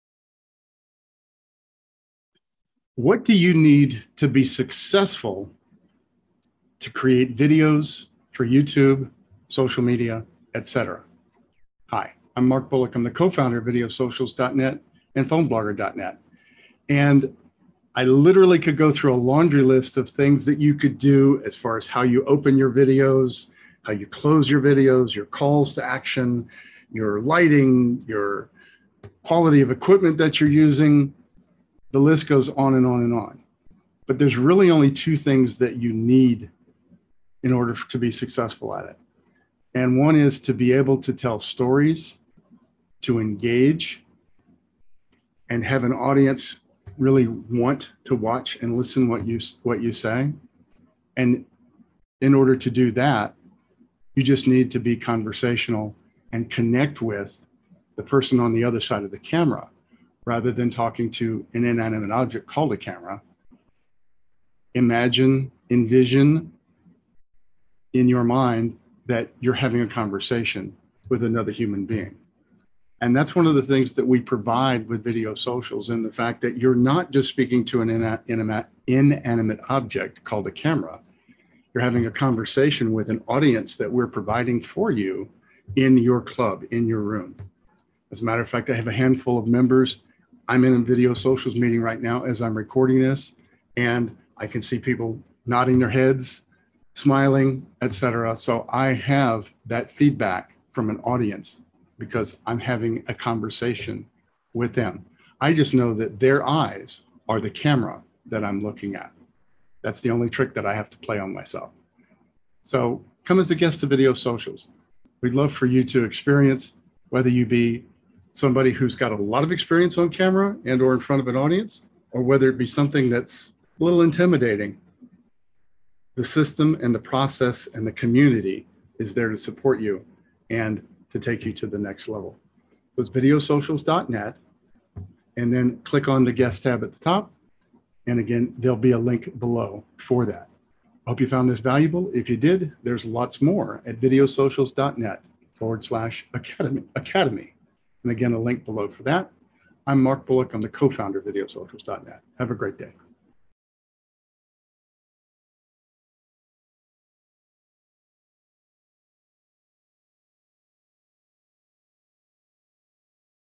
Mark Bullock: The Easiest Way to Get Started in Videosocials
The list of things to do to be successful when creating videos is very long, but according to Mark Bullock, Videosocials co-founder and COO, there are really only two things that you need: Be a storyteller and have a conversation. Watch the video to learn more.

Get practice at marketing yourself with supportive feedback: be our guest at a Videosocials club